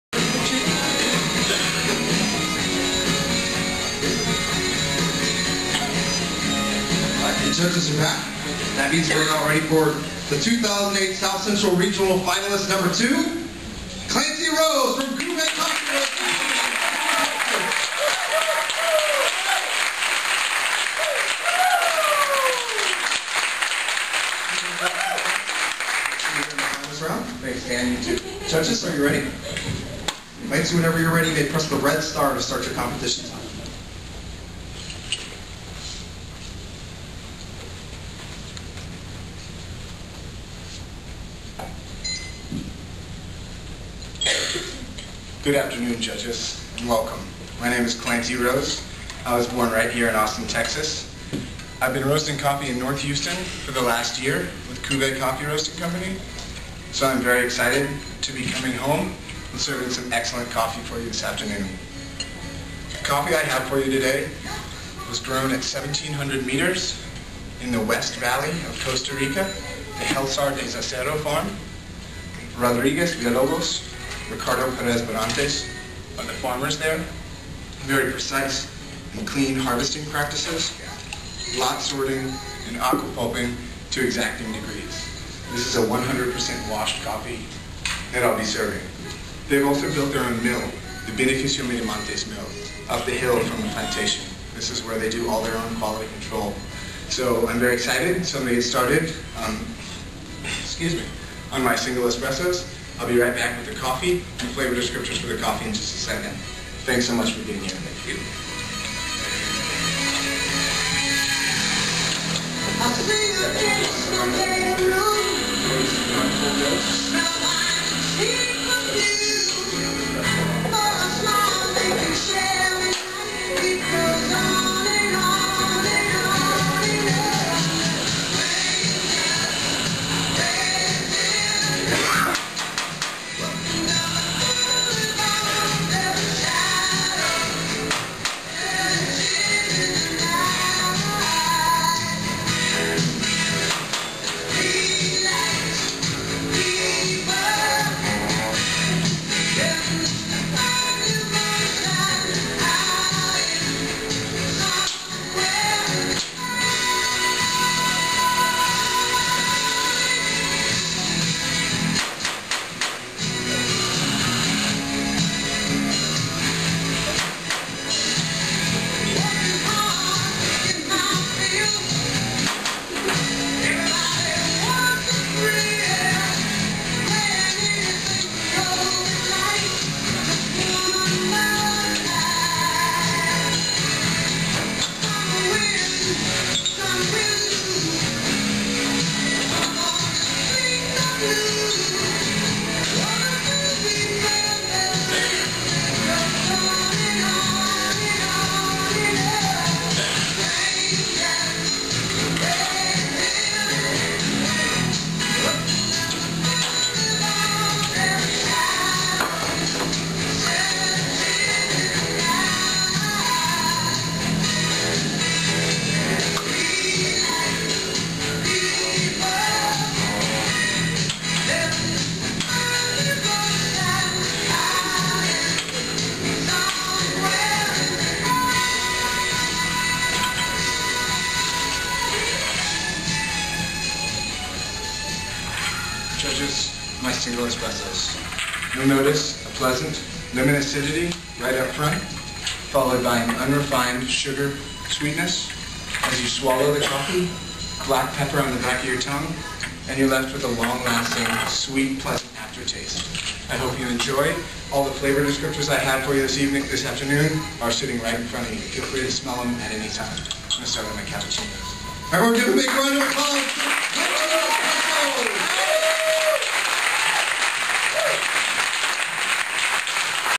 2008 SCRBC Champ's Espresso
Clancy pours espresso for the judges in the final round of the 2008 SCRBC